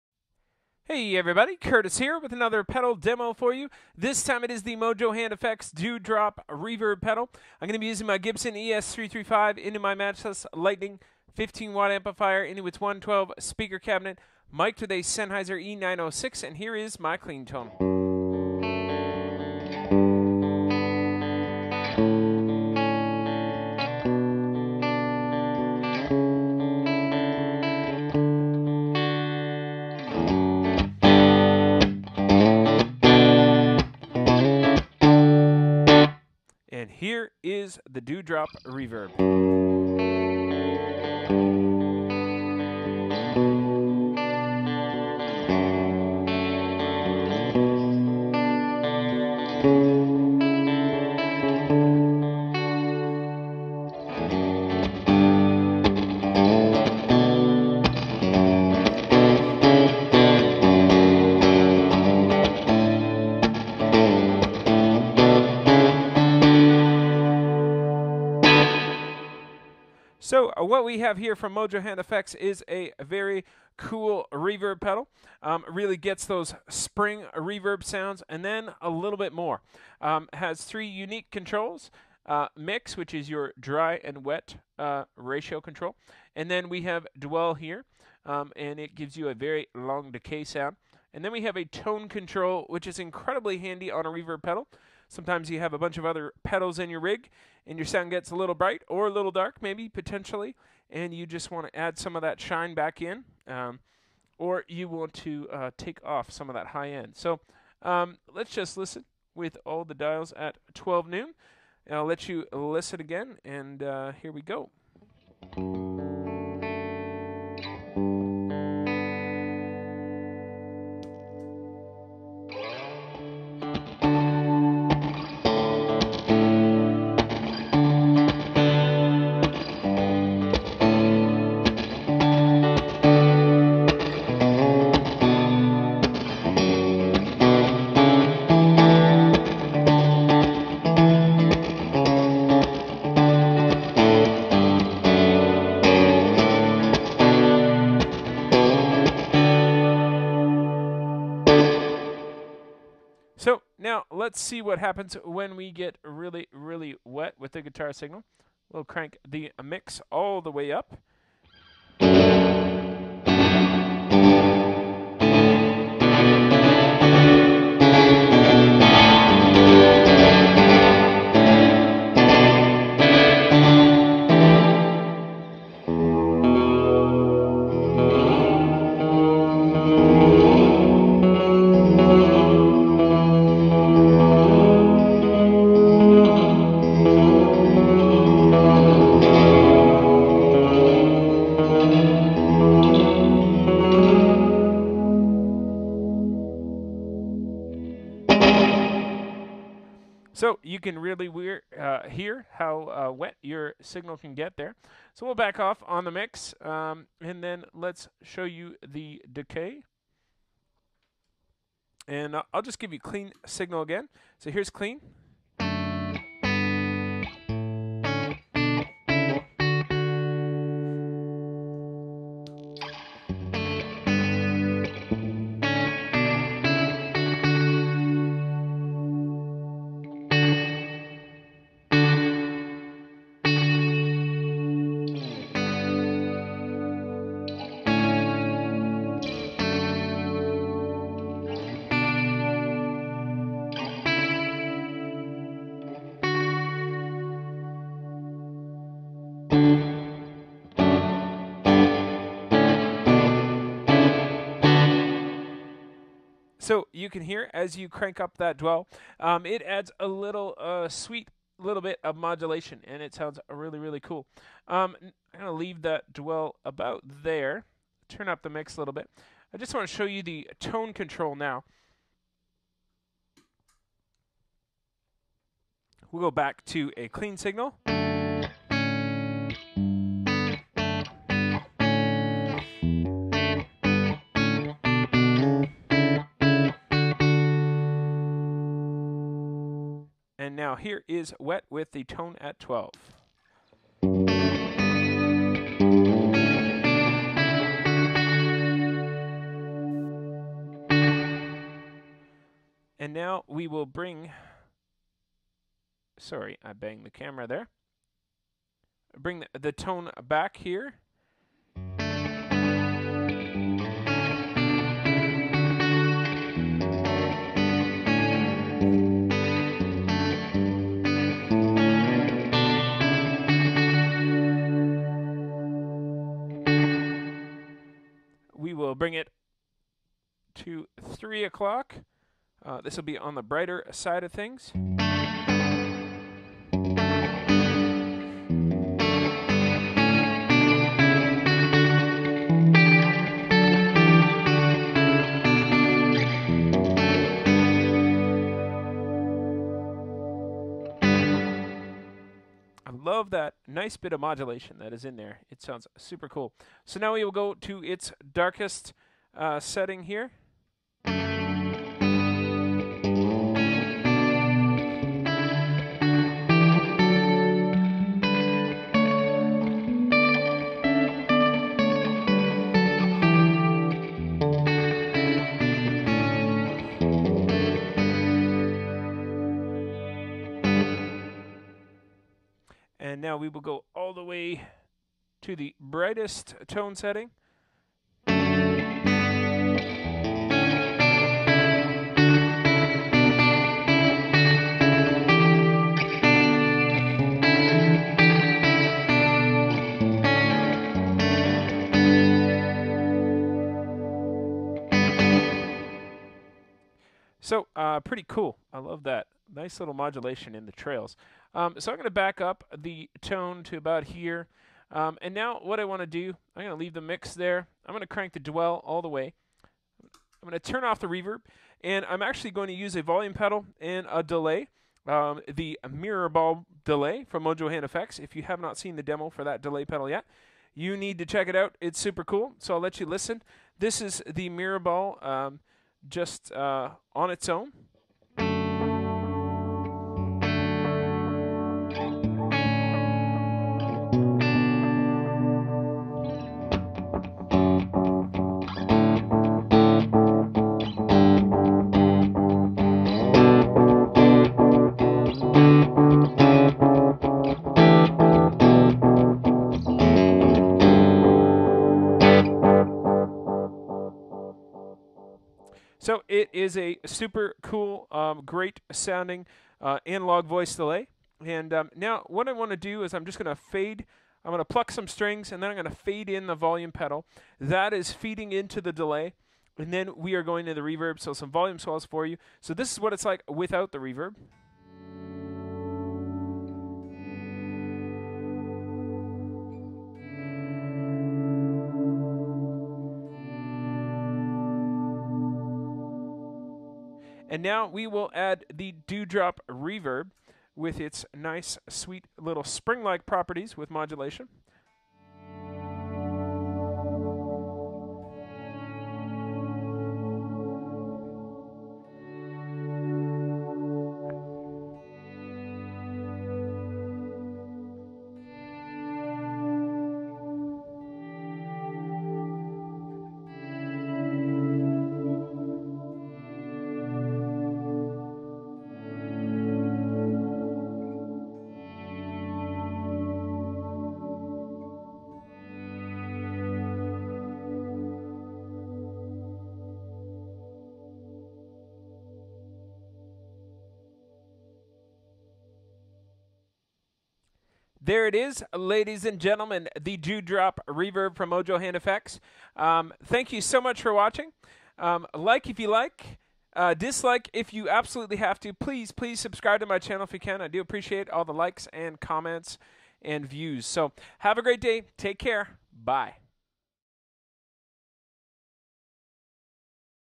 Mojohand fx Dewdrop reverb
This is a demo of the Mojohand FX Dewdrop Reverb.

"The Dewdrop excels in both versatility AND useability by offering a simple, intuitive layout. It's carefully voiced to give you 100% useable spring reverb tones ranging from subtle and damp to full on ambient wetness. It's designed to give your tone a splash of color alongside a minimal set of controls that allow multiple shades and textures.

Mojo Hand FX Dewdrop Reverb Features:

MIX -- Set low to stay shallow, crank for full soak
DWELL -- Add in your desired level of 'atmosphere'
TONE -- Cut back for warmth, up for sparkle"